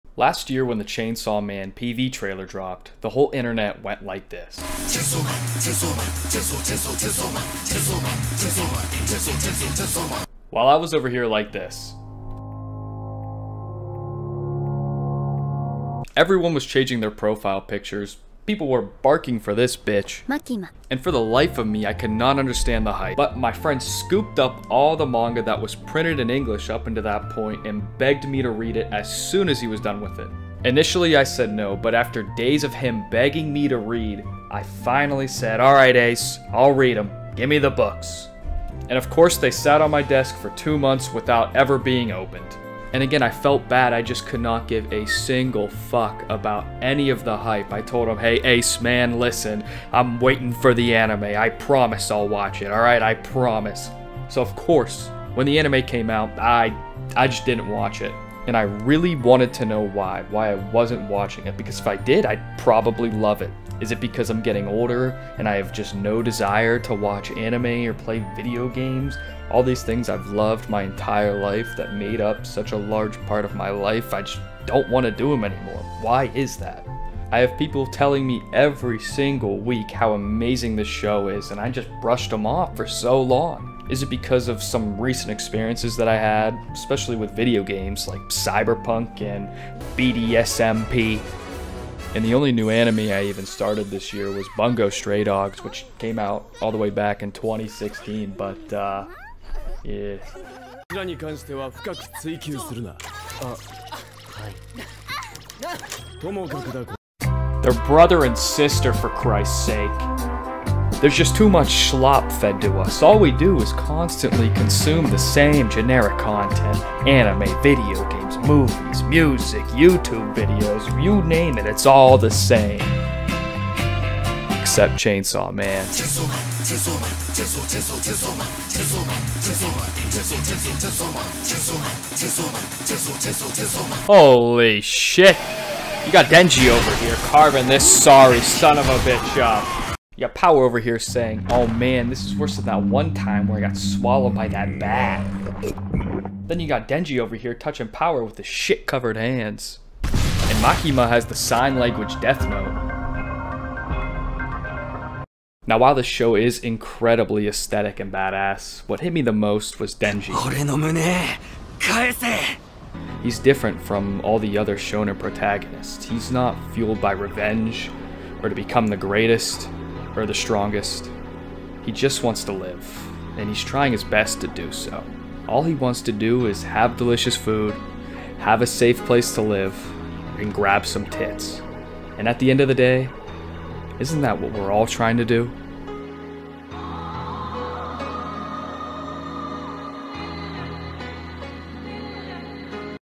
Chainsaw Man Got Me Back Into Anime
How Chainsaw Man made me come the realization that not everything is garbage shlop today.